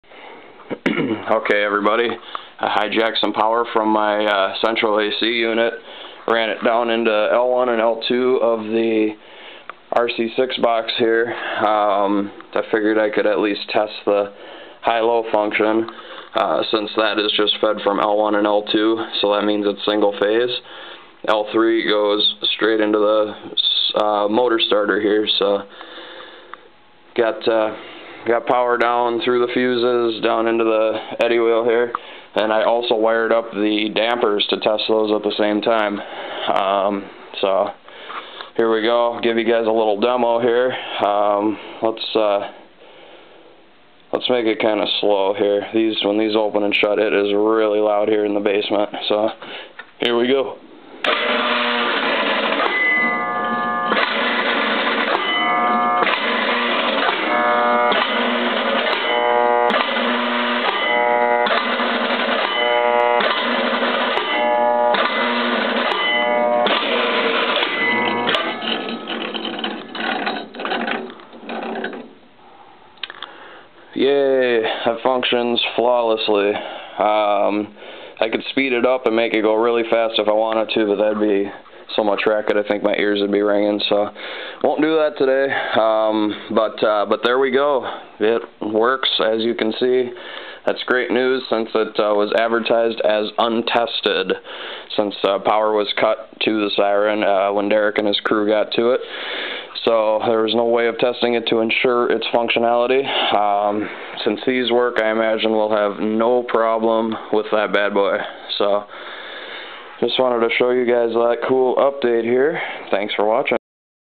Federal Signal 3T22 Tornado Siren Flasher/Solenoid Test
I decided to follow some wires in the RC6a box to see what did what, and I figured out pretty quickly that the flasher and solenoids are all 220 vac single phase, so watch and enjoy!